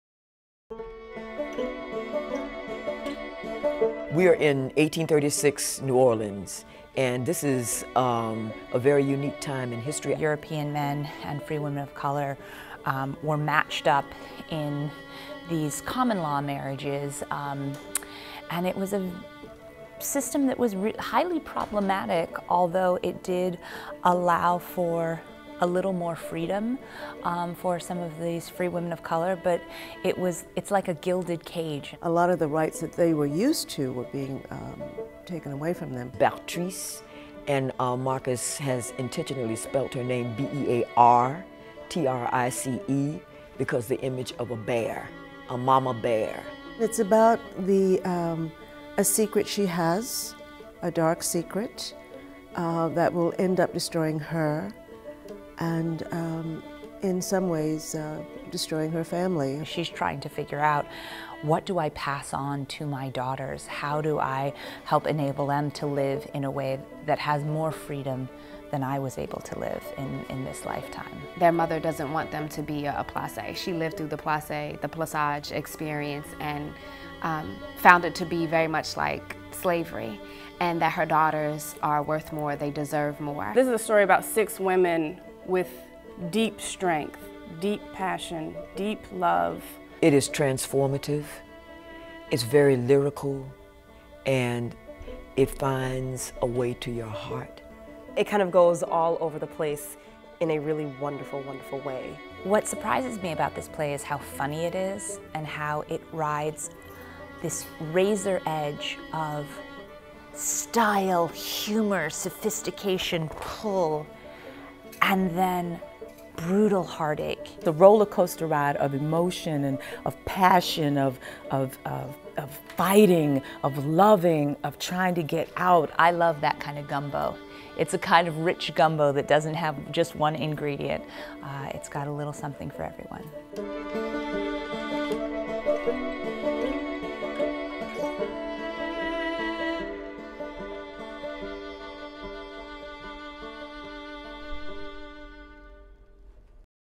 Behind the Scenes of "The House that will not Stand"
Director Patricia McGregor and cast members Joniece Abbott-Pratt, Harriett D. Foy, Lizan Mitchell, Petronia Paley, Flor De Liz Perez, and Tiffany Rachelle Stewart introduce Marcus Gardley's "The House that will not Stand."

Yale Repertory Theatre, World Premiere, April 18-May 10.